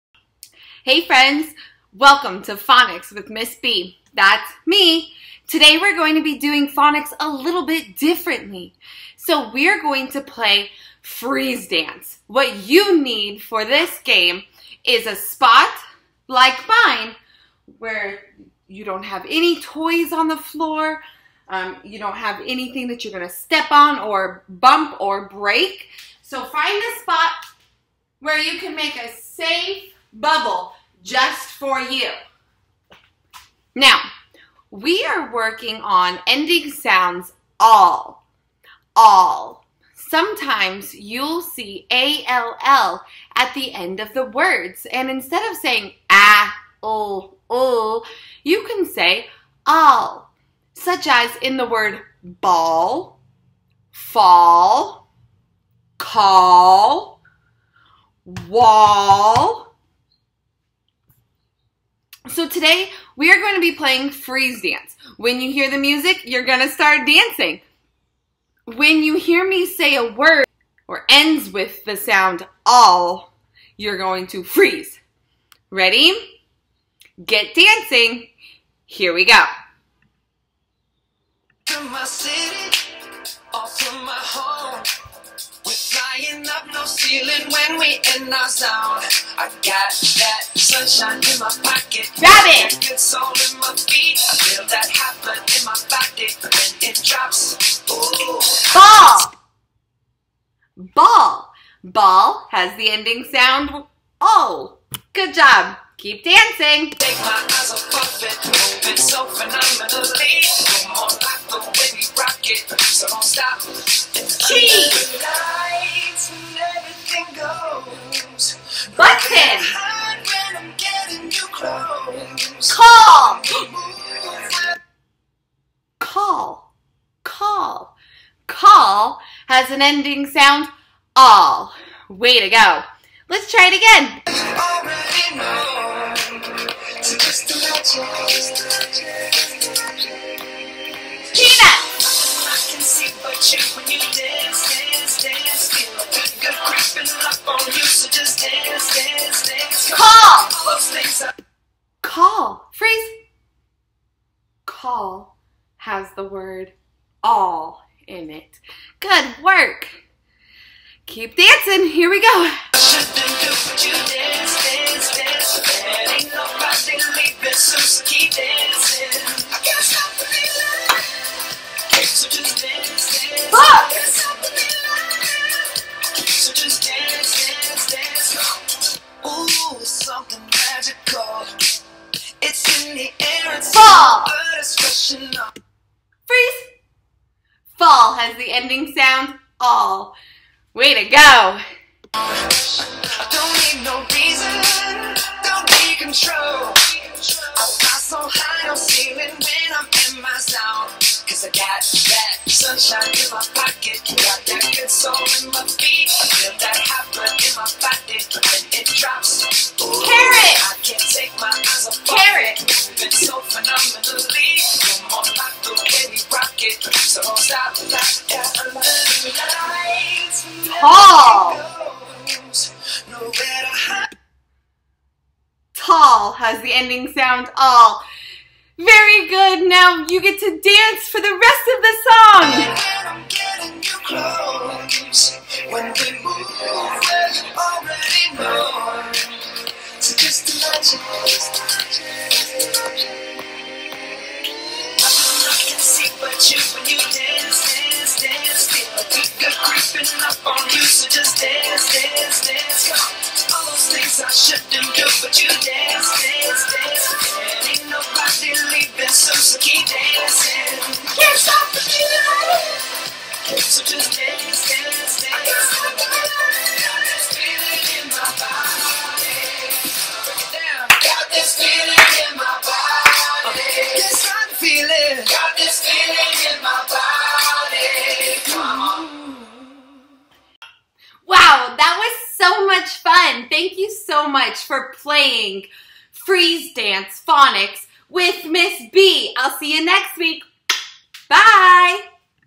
Phonics Freeze Dance With Miss B
Get your body moving while learning about ending sounds -all.